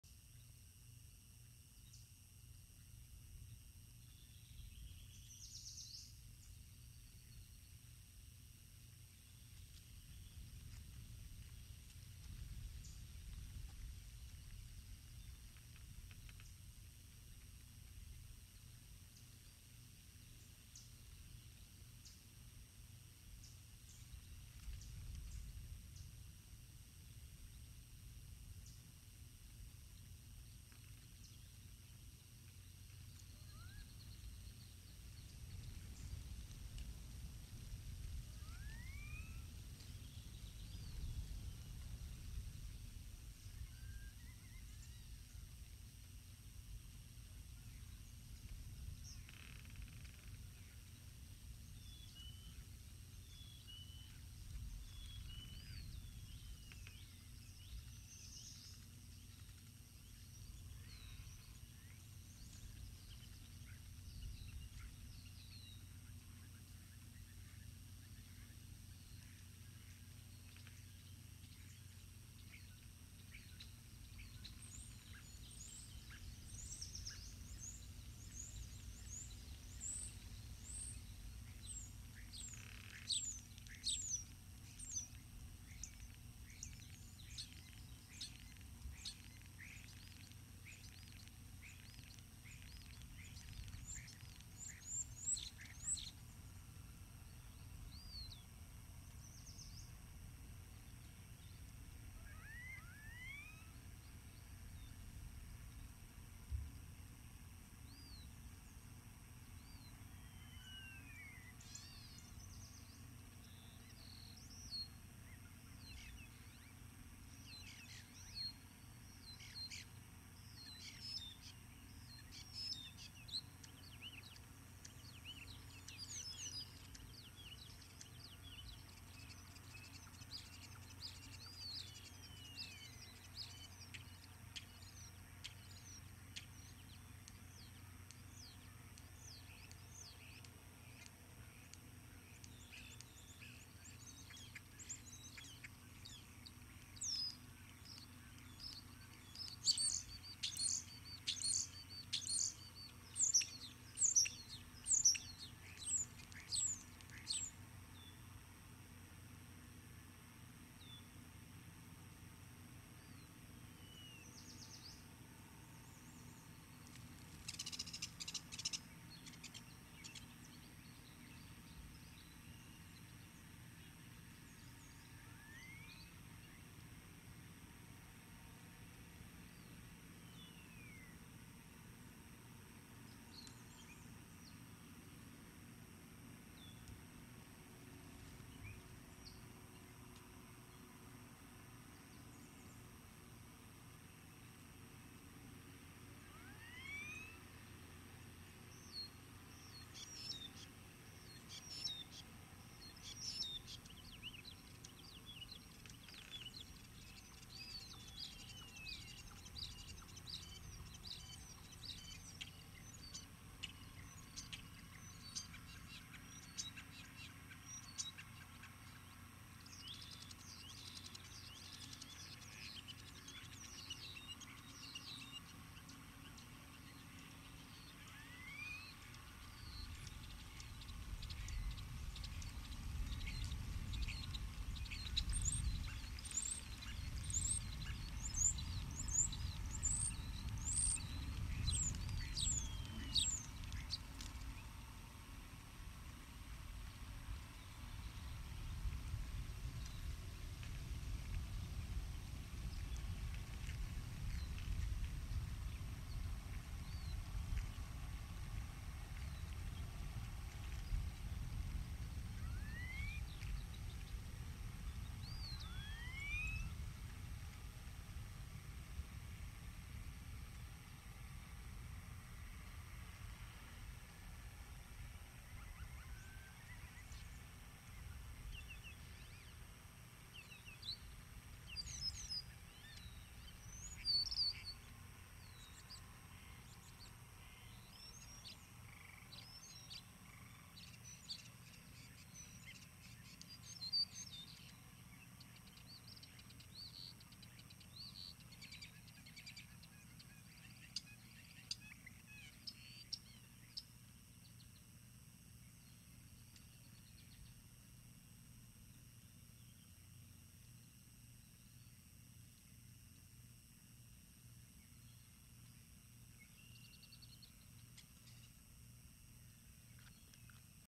Decorah North 6-10=24 DN17 (L) gets restless on left side perch
Parents were perched on T3, wondering if the kids will join them